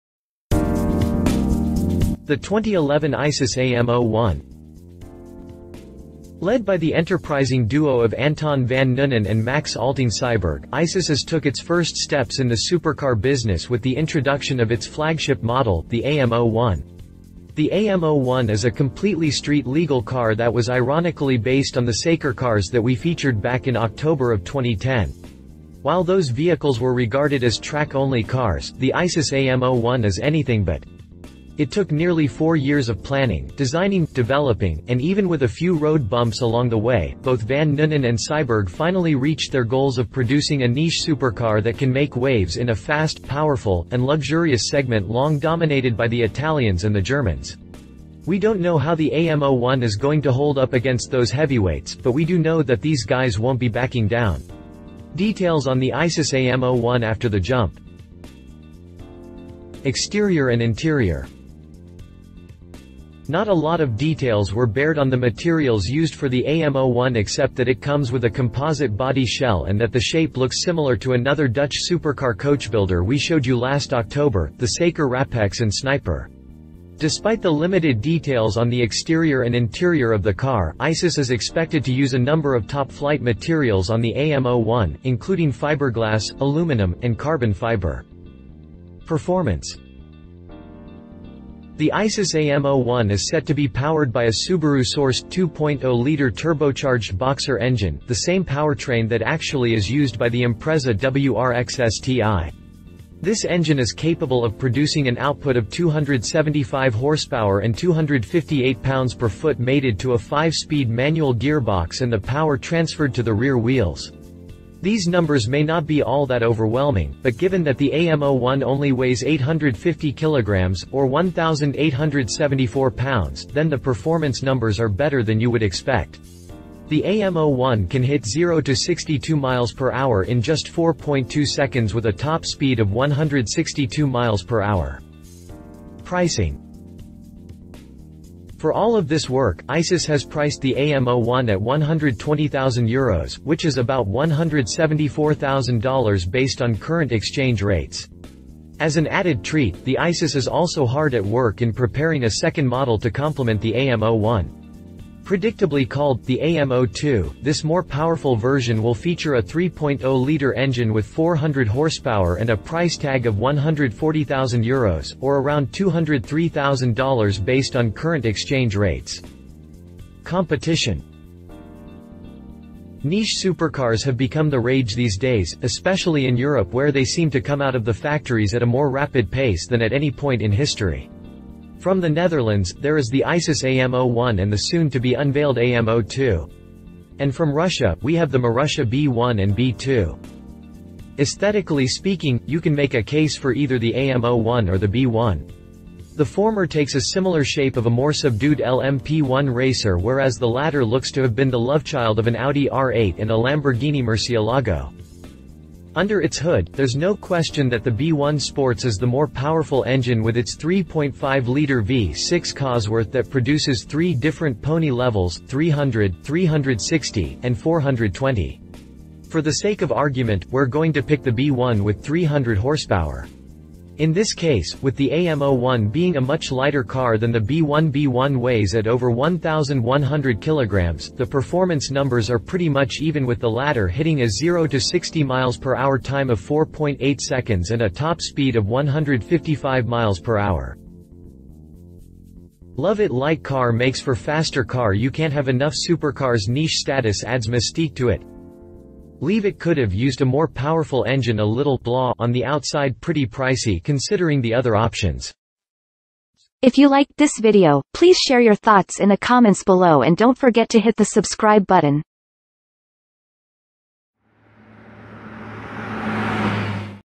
2011 ISIS AM01
Led by the enterprising duo of Anton van Nunen and Max Alting Siberg, ISIS is took its first steps in the supercar business with the introduction of its flagship model, the AM01. The AM01 is a completely street-legal car that was ironically based on the Saker cars that we featured back in October of 2010. While those vehicles were regarded as track-only cars, the ISIS AM01 is anything but. It took nearly four years of planning, designing, developing, and even with a few road bumps along the way, both van Nunen and Siberg finally reached their goals of producing a niche supercar that can make waves in a fast, powerful, and luxurious segment long dominated by the Italians and the Germans.
We don’t know how the AM01 is going to hold up against those heavyweights, but we do know that these guys won’t be backing down.
Details on the Isis AM01 after the jump.